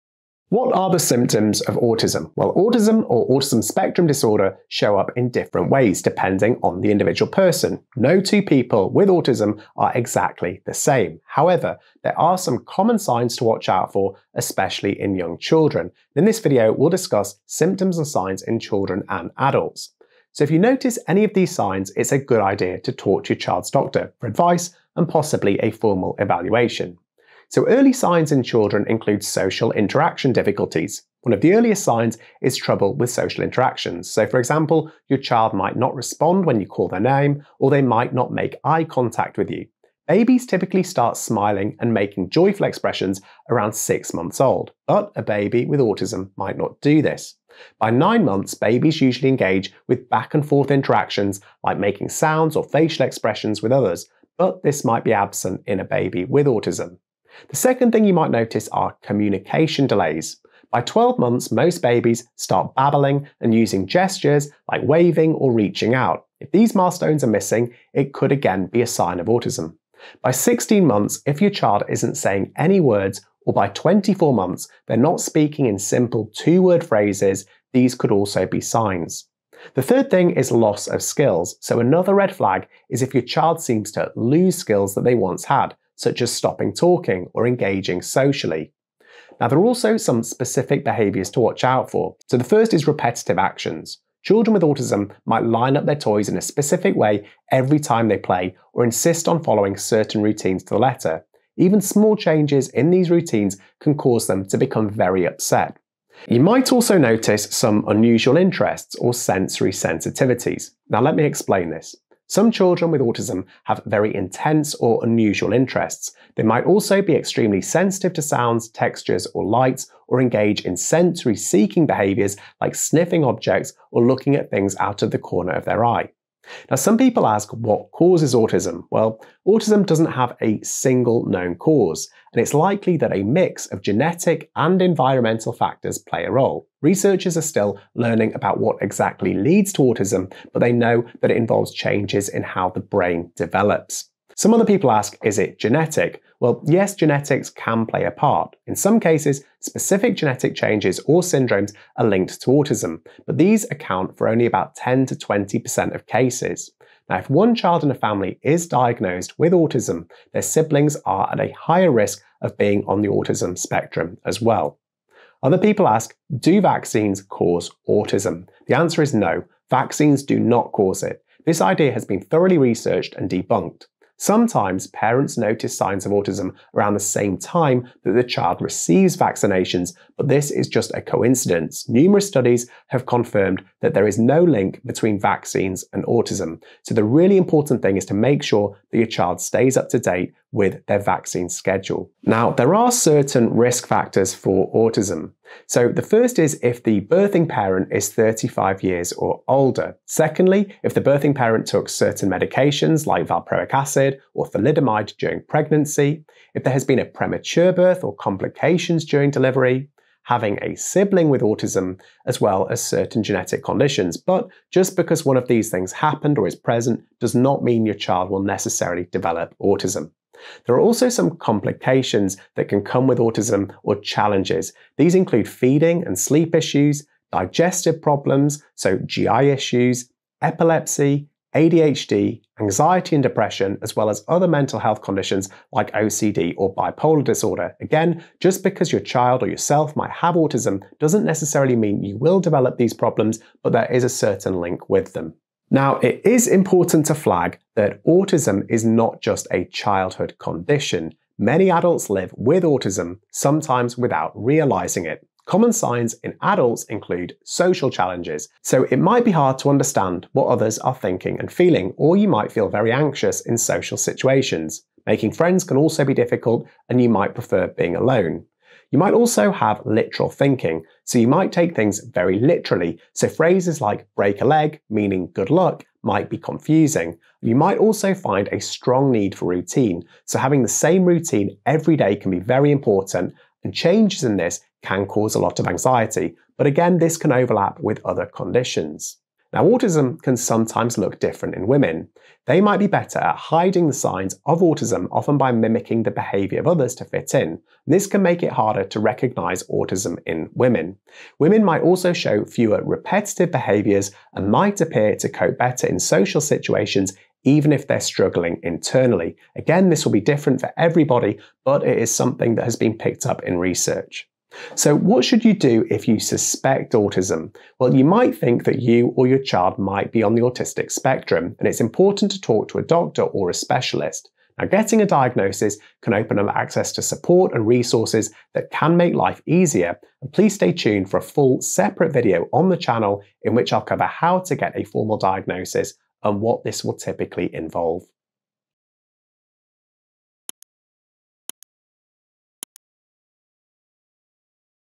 Signs and Symptoms of Autism
Doctor O'Donovan explains potential signs and symptoms of autism.

According to the NHS (UK):

Autistic people may:

- find it hard to communicate and interact with other people
- find it hard to understand how other people think or feel
- find things like bright lights or loud noises overwhelming, stressful or uncomfortable
- get anxious or upset about unfamiliar situations and social events
- take longer to understand information
- do or think the same things over and over

It is important to know that being autistic does not mean you have an illness or disease. It means your brain works in a different way from other people.

It's something you're born with. Signs of autism might be noticed when you're very young, or not until you're older.

If you're autistic, you're autistic your whole life.

Autism is not a medical condition with treatments or a "cure". But some people need support to help them with certain things.

Contents:

00:00 - Introduction
00:21 - Potential signs and symptoms of autism in children
02:06 - Causes of Autism
02:25 - Is autism genetic?
02:46 - Do vaccines cause autism?
03:12 - Risk factors for autism
03:40 - Complications
04:06 - Autism signs and symptoms in adults
04:50 - Autism in women
05:15 - What to do if you suspect autism?

Video made on: 1 September 2024
Next update: 1 September 2027